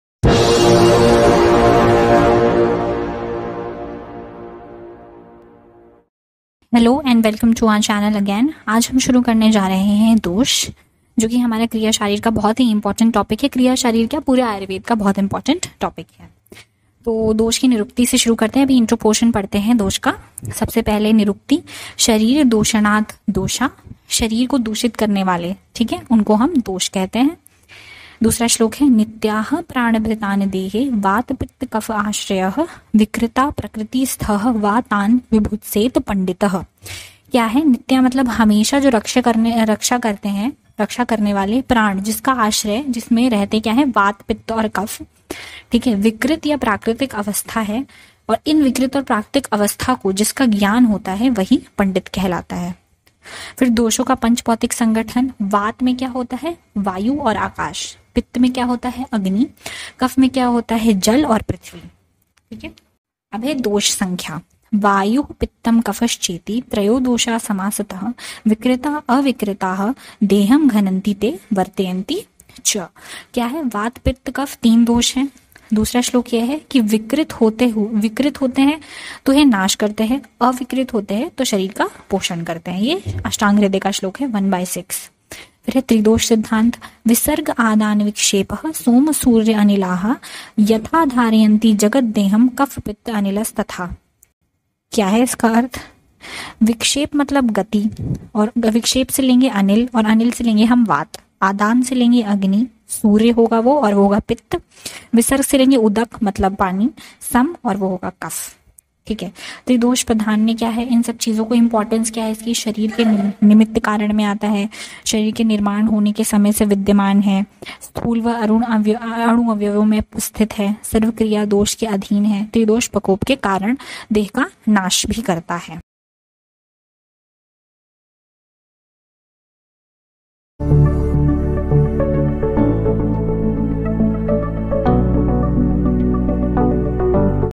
Dosha ( दोष ) General introduction , Vyutpatti and Nirukti in Hindi | BAMS 1st Year Notes
Welcome to Ayurved Studies

There are three doshas in Ayurveda
 vata, pitta, and kapha.

🌿 Dosha
• General Description of Tridosha
• Vyutpatti
• Nirukti
• more..

🌿 PDF Notes Download ⬇️
more update ➡️

BAMS 1st Year notes in Hindi
BAMS 1st Year lectures
Dosha Notes in Hindi
BAMS Notes in Hindi
Ayurved notes
ayurved